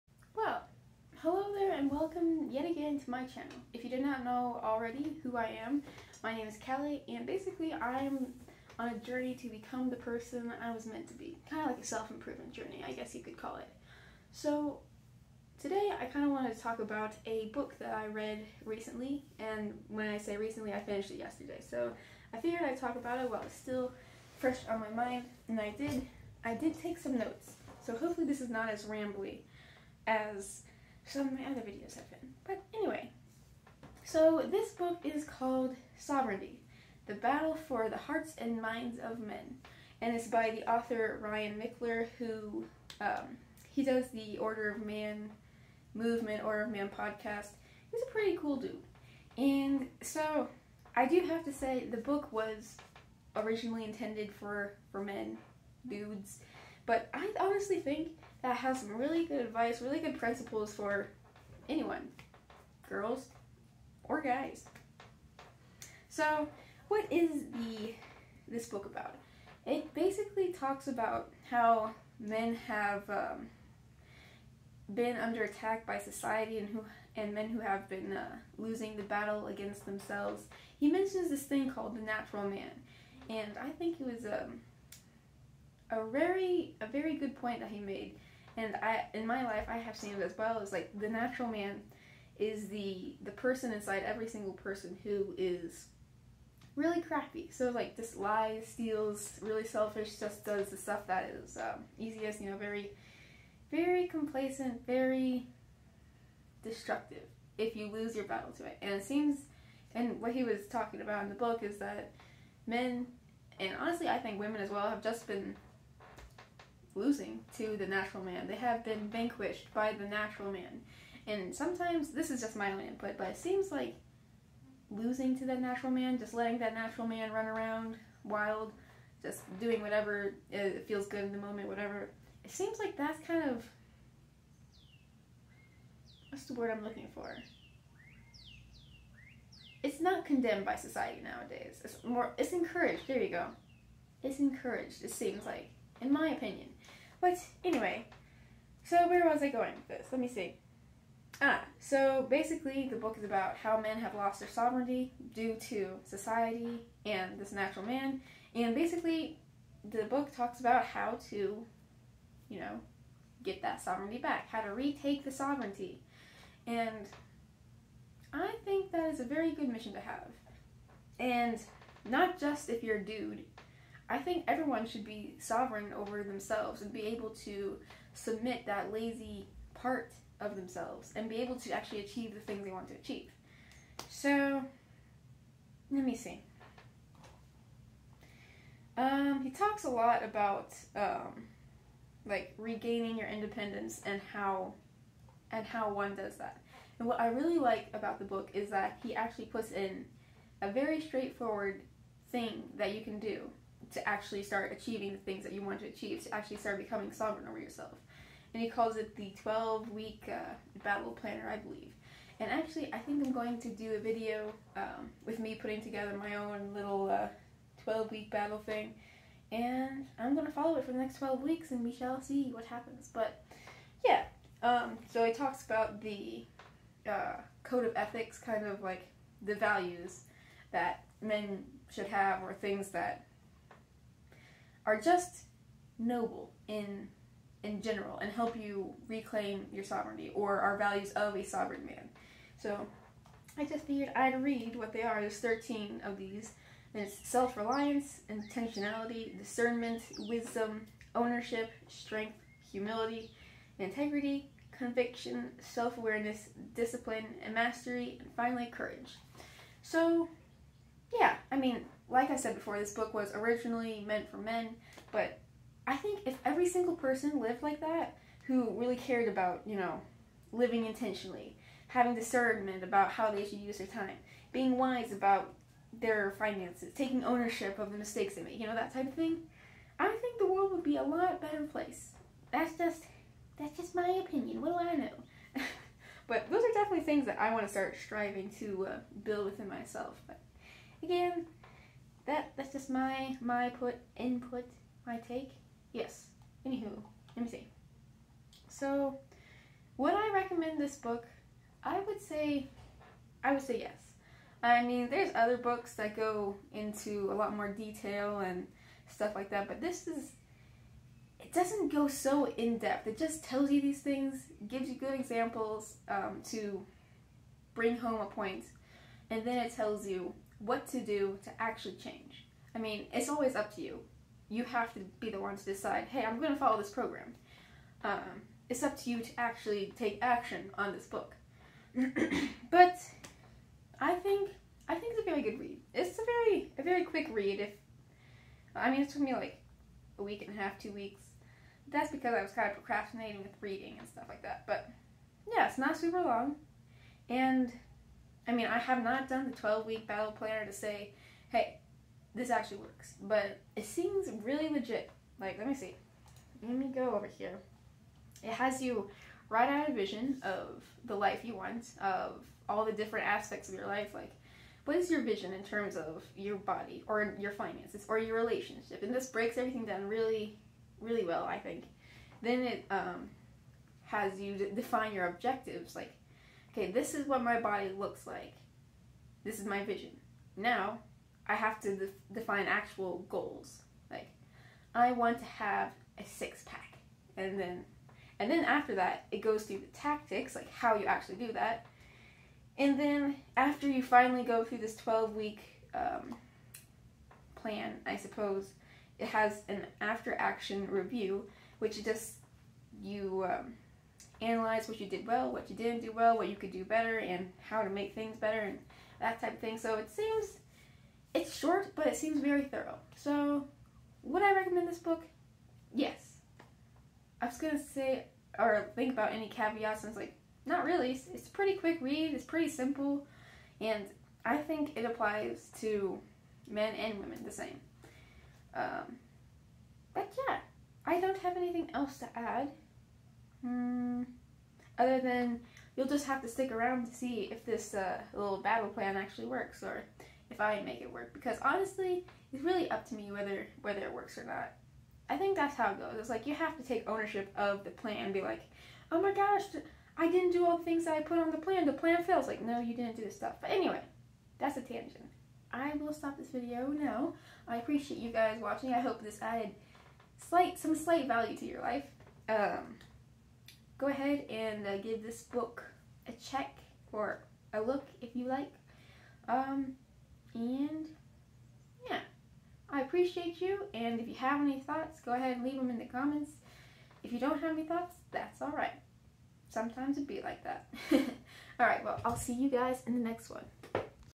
Book review and thoughts on Sovereignty: The Battle for the Hearts and Minds of Men by Ryan Michler
Hello once again!
Today I give my thoughts on Ryan Michler's book, Sovereignty: the battle for the hearts and minds of men. To sum it up: I definitely think you should check it out.